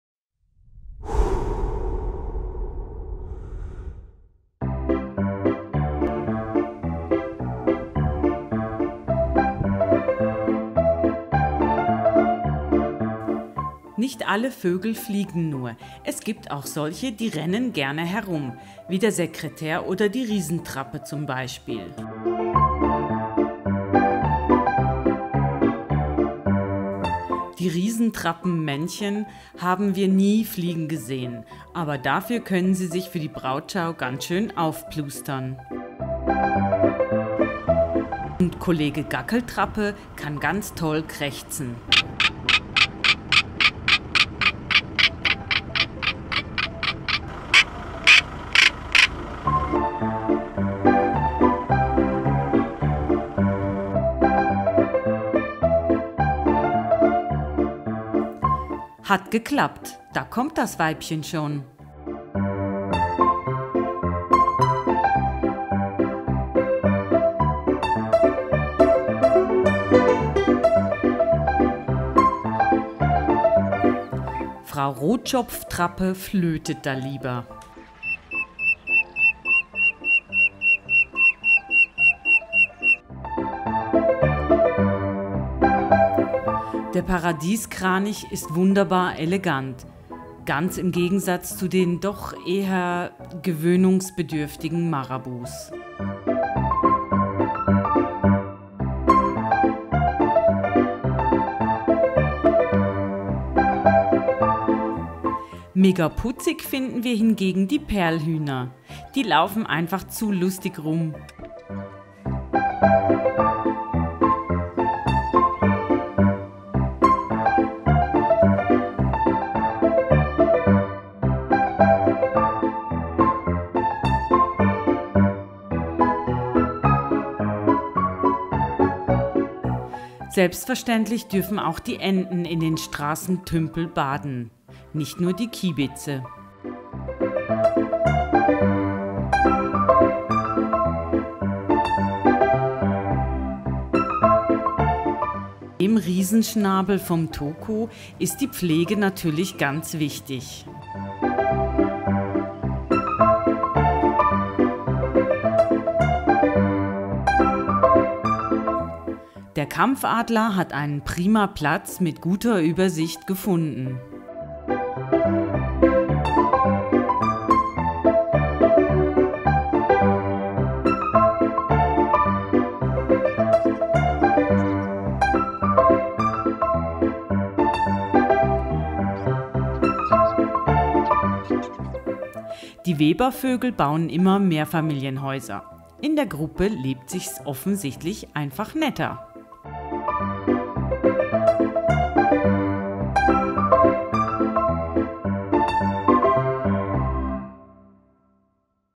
Etoscha Pfanne 5 von 5: Die Flieger • Weltreise
Die meisten Vögel, die wir in der Etoscha Pfanne beobachteten, war zu Fuss unterwegs. Sekretär, Riesentrappe, Gackeltrappe, Waffenkiebitz, Rotschopftrappe und Paradieskranich können sehr grazil durch die Steppe stolzieren, dagegen sehen die Perlhühner doch eher etwas unbeholfen und lustig aus. Damit der Gelbschnabel Toko seinen Schnabel tragen kann, braucht er vermutlich Nackenmuskeln wie ein Stier. Die Marabus gefallen uns nicht wirklich und die einzigen, die wir wirklich fliegen sahen waren Kampfadler, Webervögel und Enten.